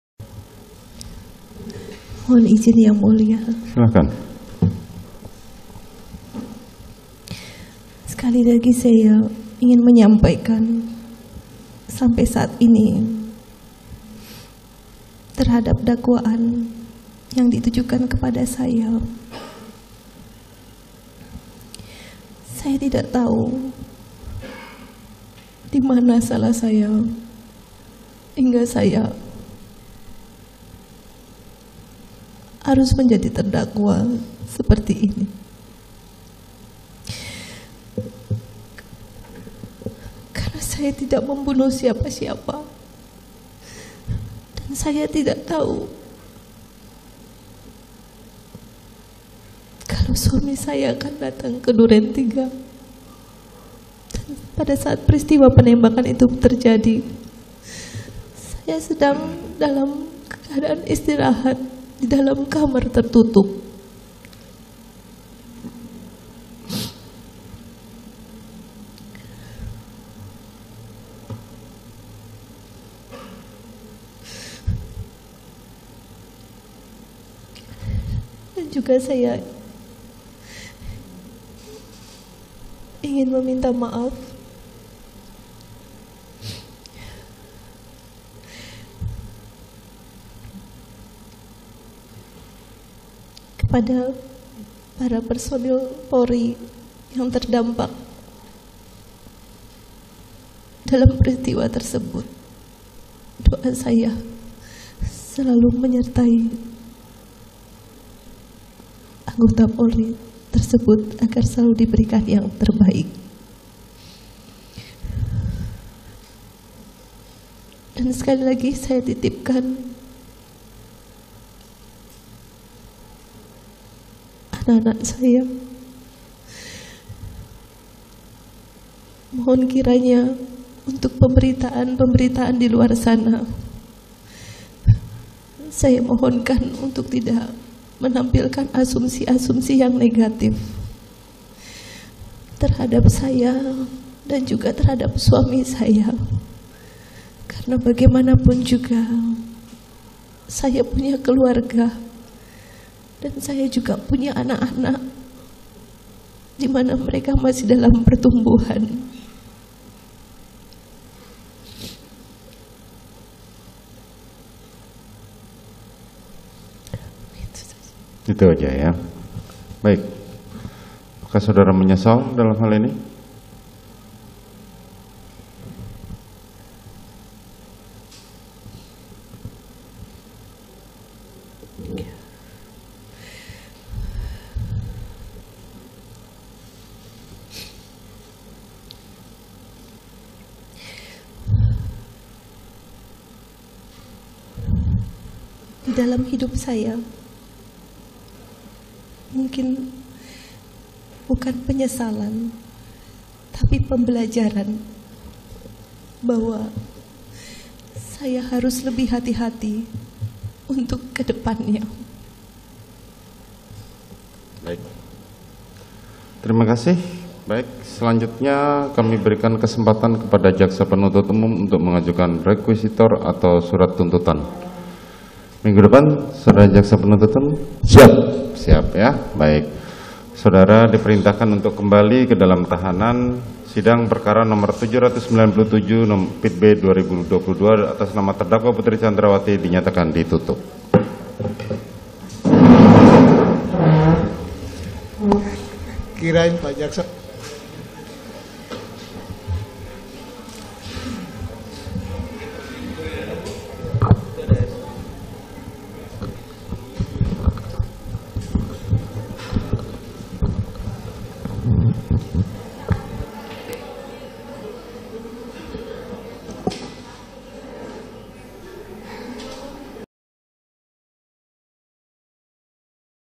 Sidang Tuntutan Putri Candrawathi Digelar 18 Januari 2023
JAKARTA, KOMPAS.TV - Terdakwa Putri Candrawathi bakal mendengarkan pembacaan surat tuntutan dari Jaksa Penuntut Umum (JPU) pada Kejaksaan Negeri (PN) Jakarta Selatan pada Rabu (18/1/2023) pekan depan.

Putri Candrawathi bakal dituntut atas kasus pembunuhan berencana terhadap Nofriansyah Yosua Huatabarat atau Brigadir J.

Hal itu disampaikan Ketua Mejelis Hakim Wahyu Iman Santoso sebelum menutup persidangan Putri Candrawathi dengan agenda pemeriksaan terdakwa.